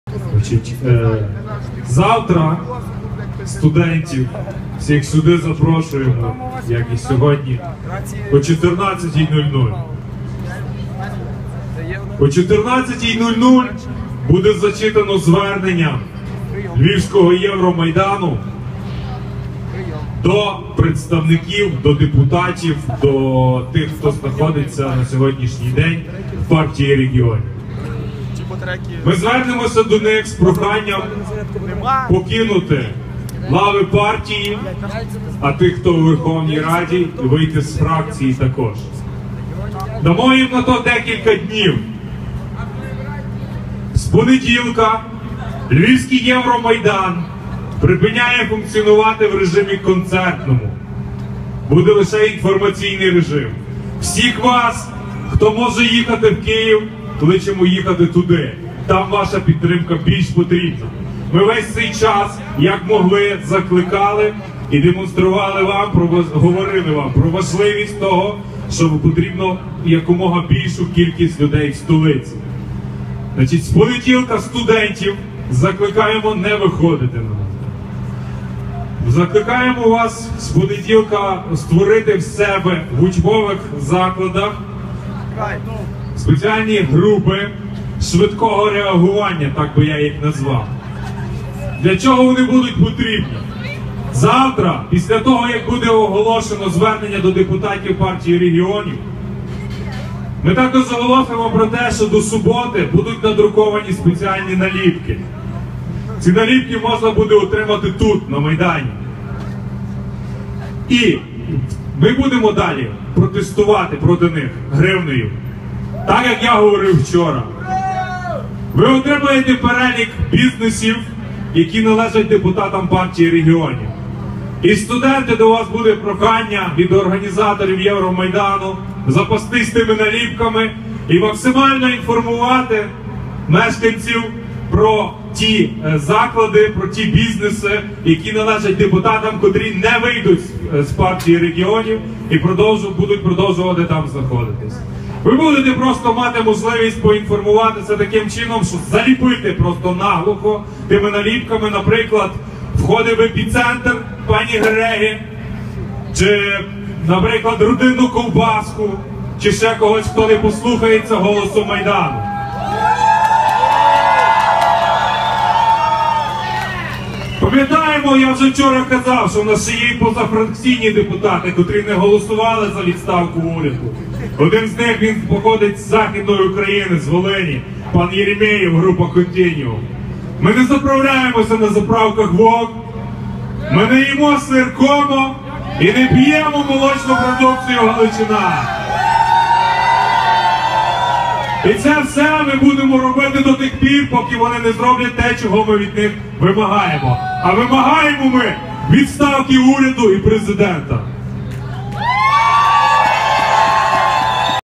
оголошує бойкот
Роман Матис анонсує акцію з бойкоту бізнесів депутатів регіоналів.